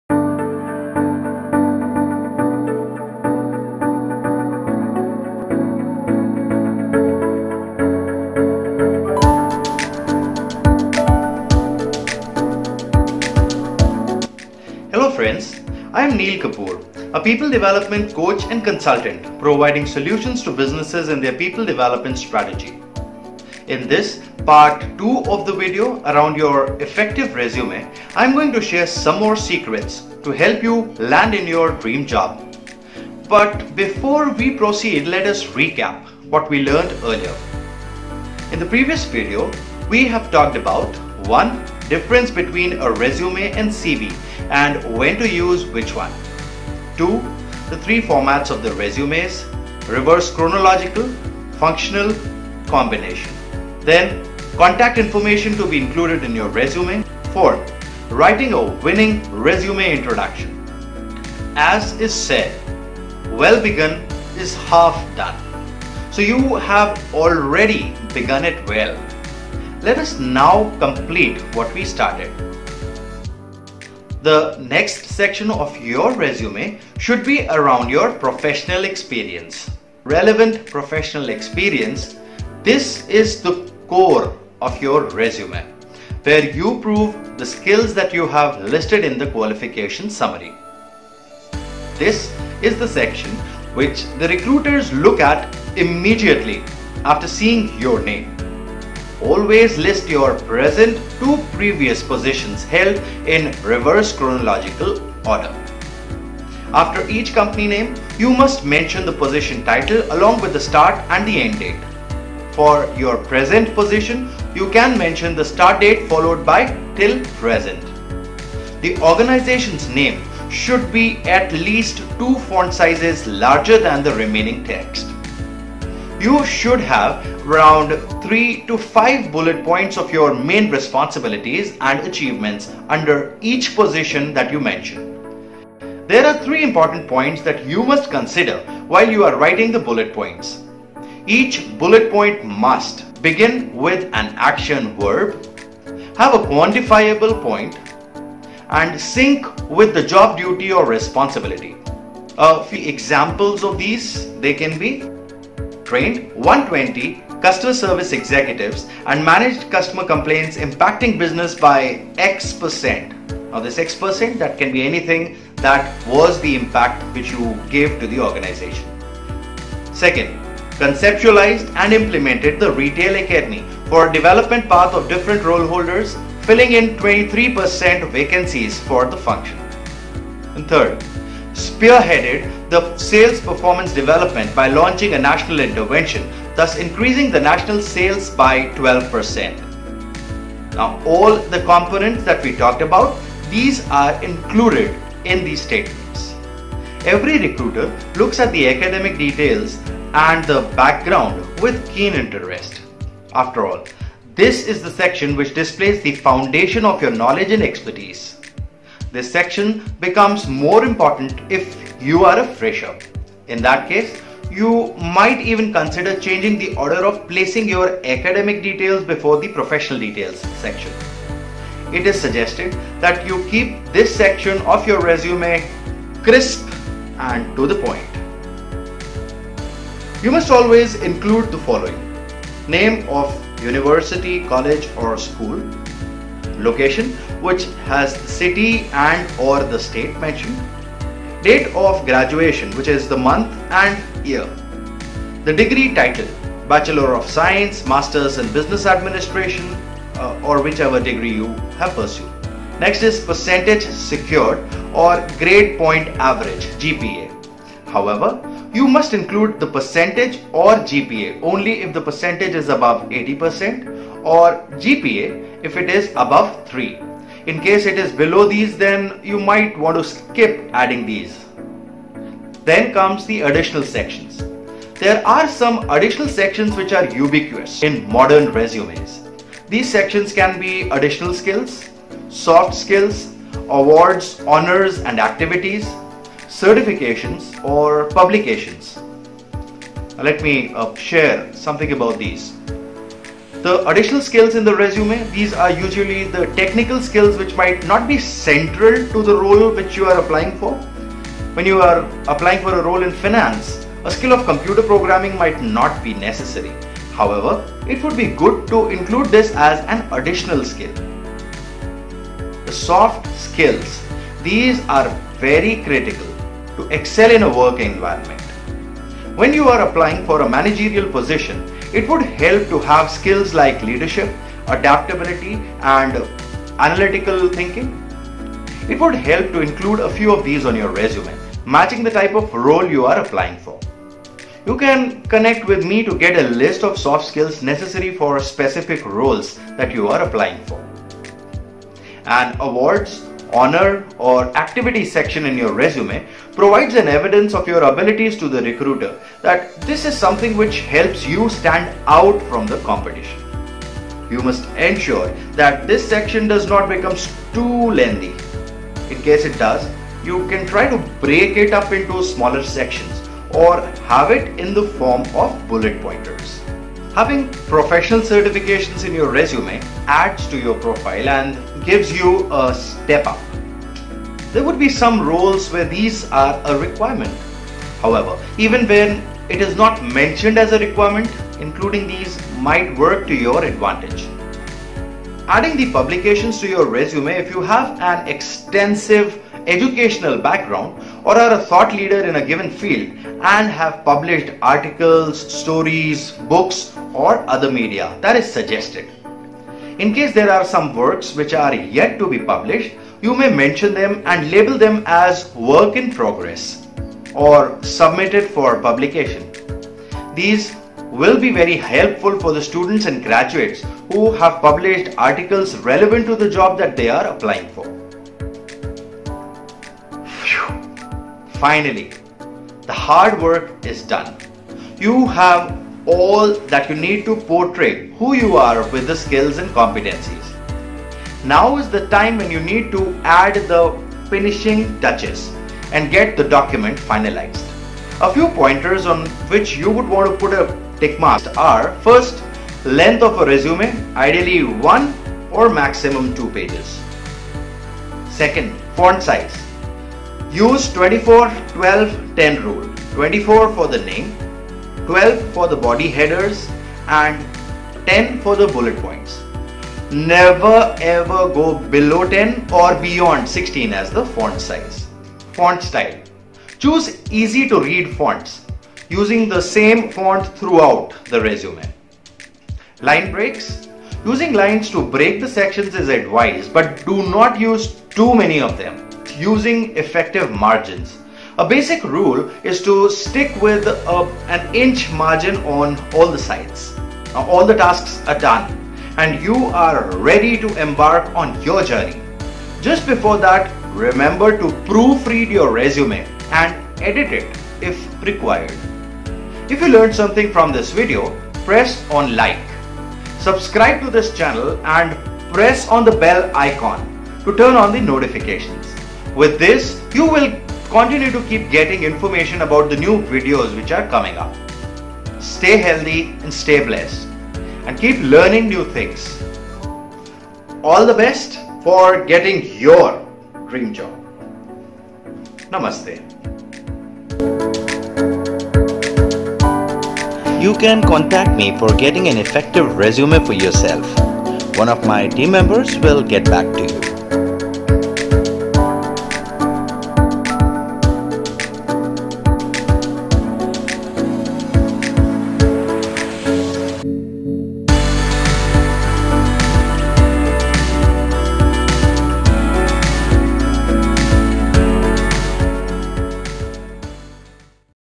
How to Write a Resume for Freshers & Experienced?: Secrets step to land in your dream job - Part 2
The first step to find a job is to start with the document that helps you get shortlisted for the role that you are applying for. With thousands of applicants for a few positions, how do you ensure that you catch the eyes of the recruiter?

There are many things which you need to look at. I covered a few ingredients of preparing a winning resume in part 1 This video, or Part 2 focuses on the remaining secret ingredients of building an effective resume. It provides you with details around putting in your Professional Experience to have the maximum impact, Academic Details, the Additional Sections like Soft Skills, Additional Skills, Awards, Certifications, etc. and the finishing touches required.

How to build a resume? How to Write a Resume for Freshers & Experienced?